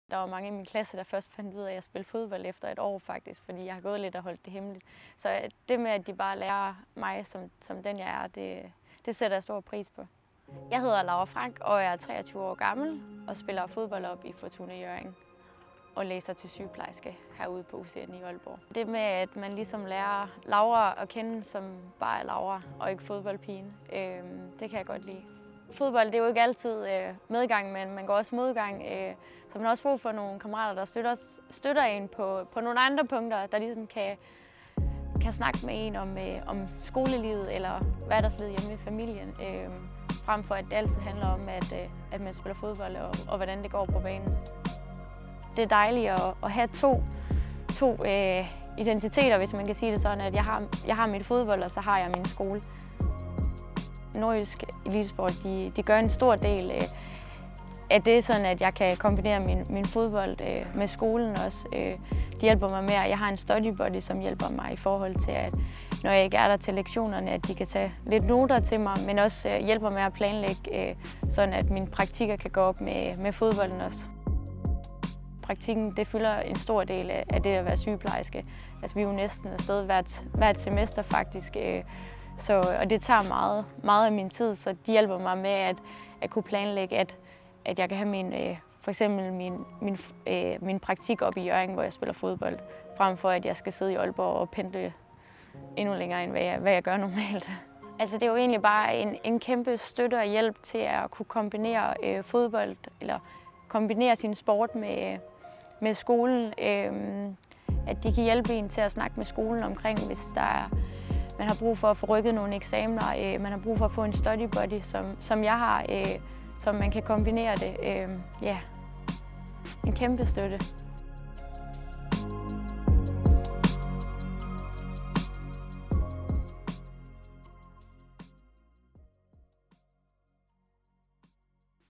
Laura Frank om at kombinere sport og uddannelse. GÅ ALL IN - også på din fremtid!
Nordjysk Elitesport hjælper til at kombinere elitesport og uddannelse på Aalborg Universitet og UCN.

Fodboldspiller, Laura Frank, fortæller om, hvorfor hun læser til Sygeplejerske på UCN ved siden af sporten, og hvordan hun får puslespillet til at gå op.

GÅ ALL IN - også på din fremtid!